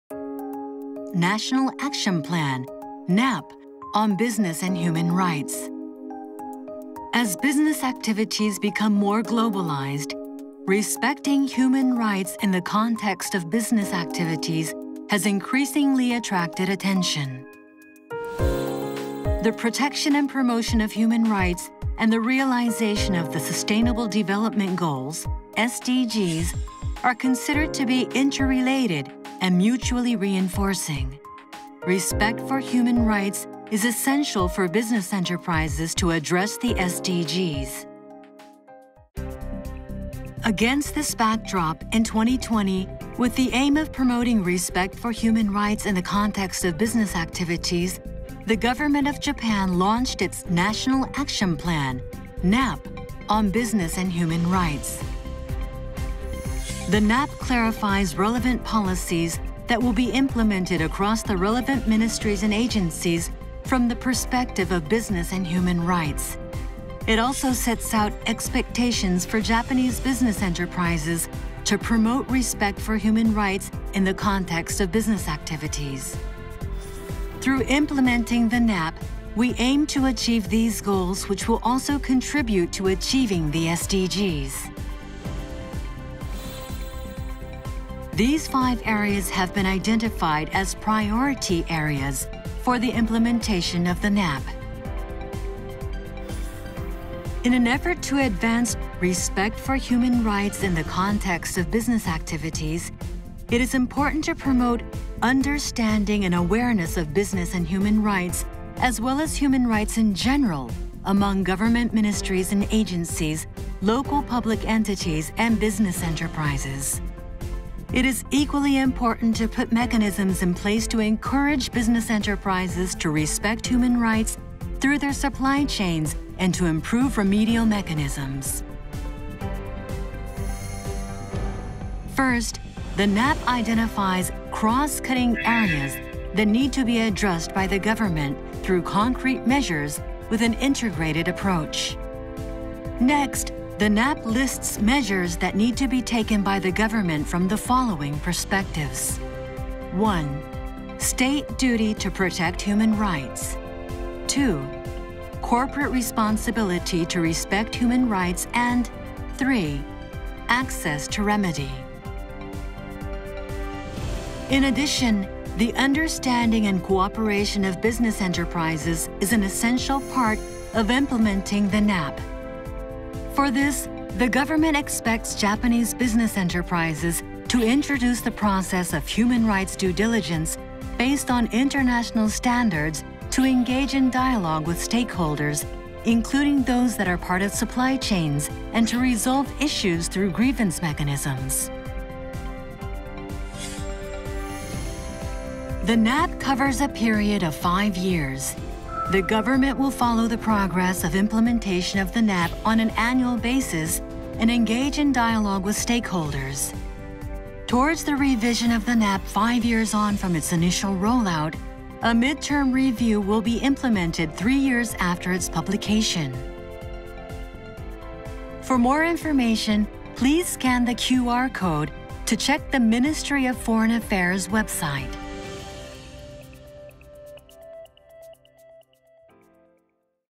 Japan's National Action Plan (NAP) on Business and Human Rights(2020-2025)
In October 2020, with the aim of promoting respect for human rights in the context of business activities, the Government of Japan launched a National Action Plan (NAP) on Business and Human Rights.